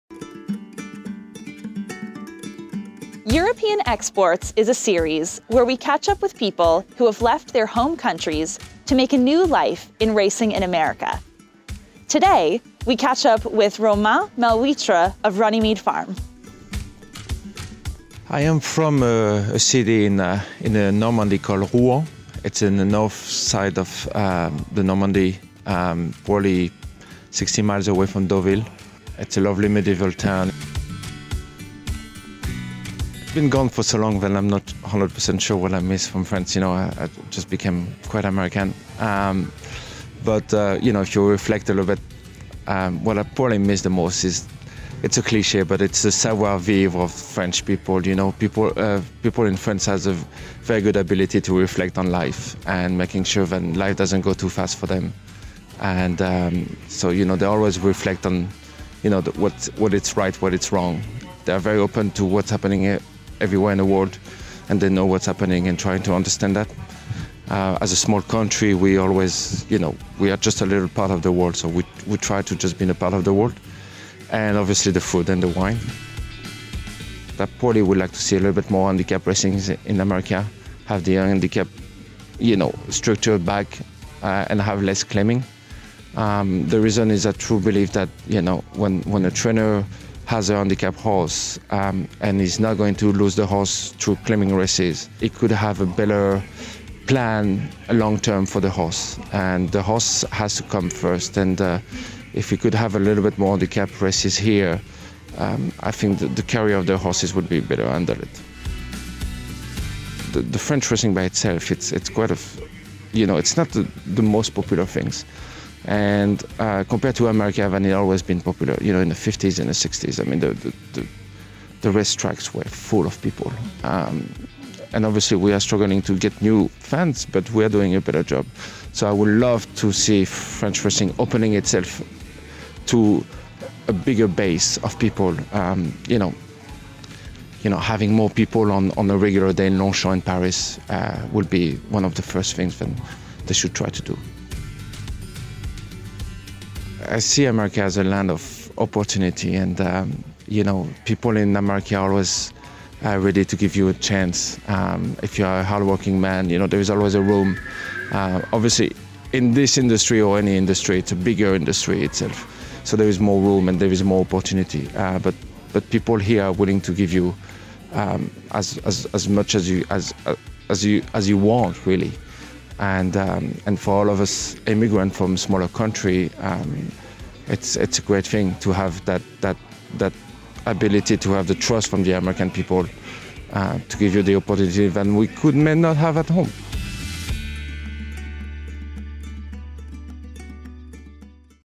Romain Malhouitre: European Exports, with Kelsey Riley
Romain Malhouitre came to the US from France 15 years ago. Today, he serves as the general manager of Runnymede Farm, and he talks with Kelsey Riley about what he appreciates about America, and misses about France.